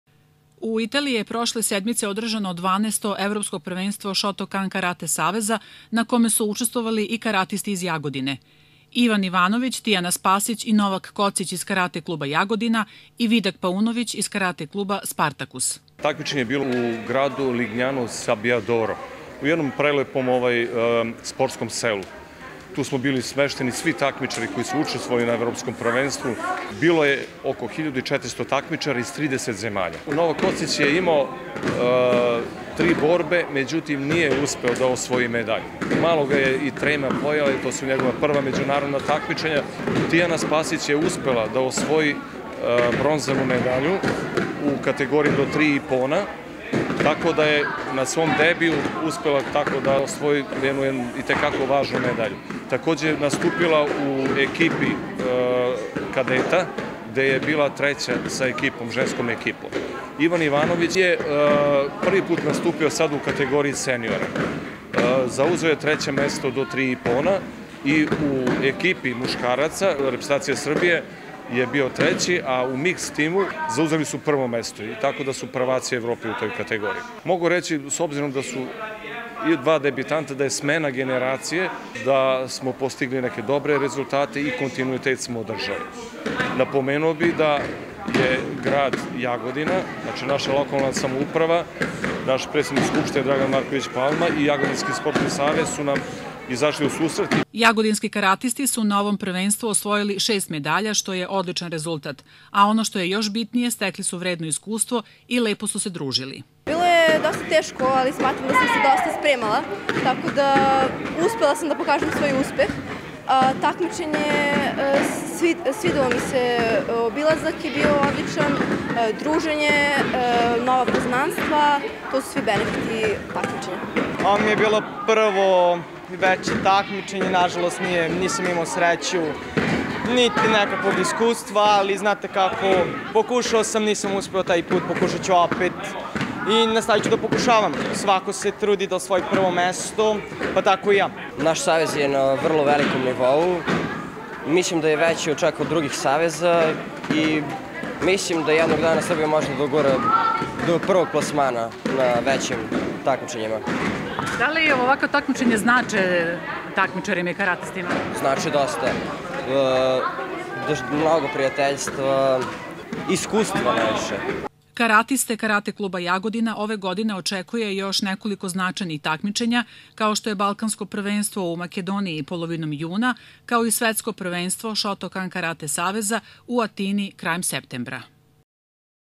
Info - Uspesi jagodinskih karatista u Italiji (TV KCN 06.04.2023)
Na Evropskom prvenstvu šotokan federacije u Italiji, prošle sedmice, učestvovalisu i kartisti iz dva jagodinska karate kluba. Četiri takmičara su osvojila šest medalja.

reporter Jelena Lukić
snimatelj Toplica Antić
montaža Dušan Mitrović